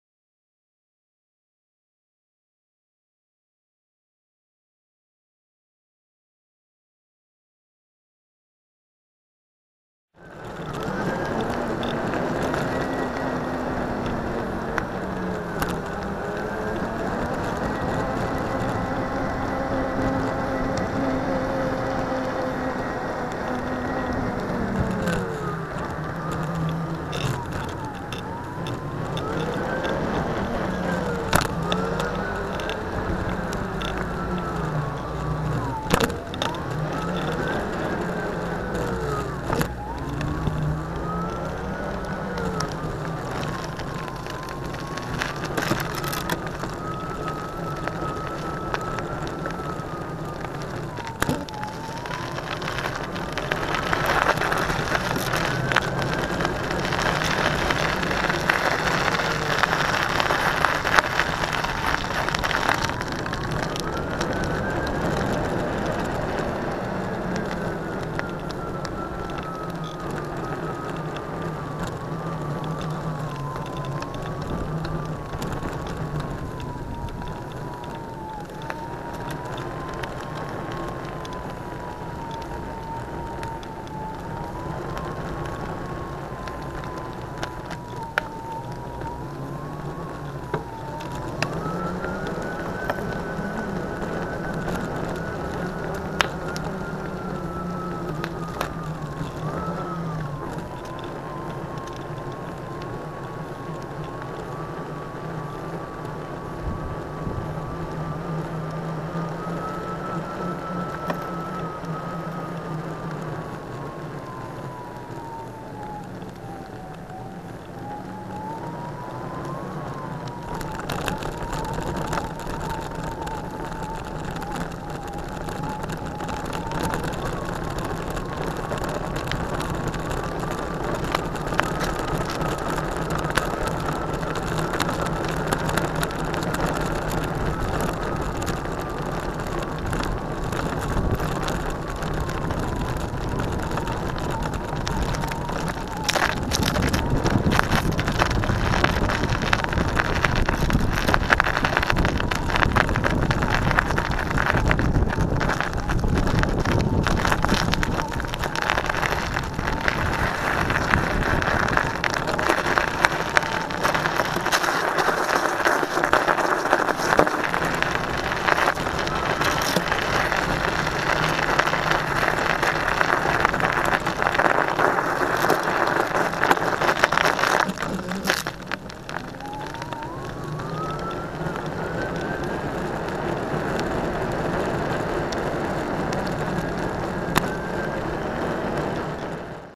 Riding the Solowheel through Kingsfield Abbey
after crossing two pedestrian crossings I ride through the cloister.
Some people fly across the big pond to see this abbey - you get a free view here ;-)